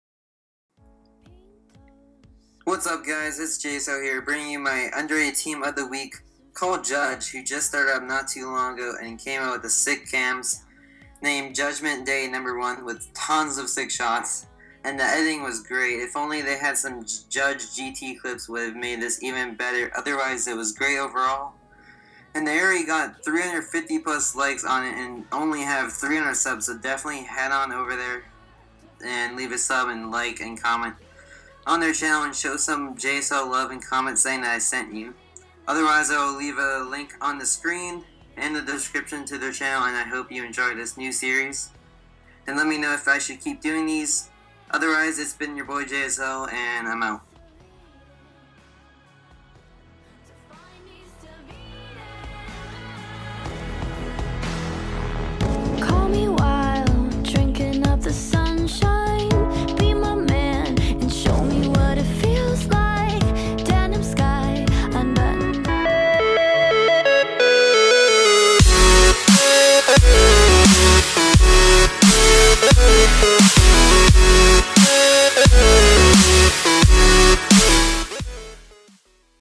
JSL : Underrated Team Of The Week #1
LETS SMASH 100 LIKES GUYS! =)

- Open -

aye guys , bringing you "Underrated Team of the week #1" coming at you the team called Judge who just started and brought out a preety sweet cams , so definetly check them out and if you want to see more of these let me know! Also I recorded this at 3 a.m so I was very tired so thats why I sound like im about to past out otherwise I hope you enjoyed and have a nice day!

Twitter : Follow me to keep up to date!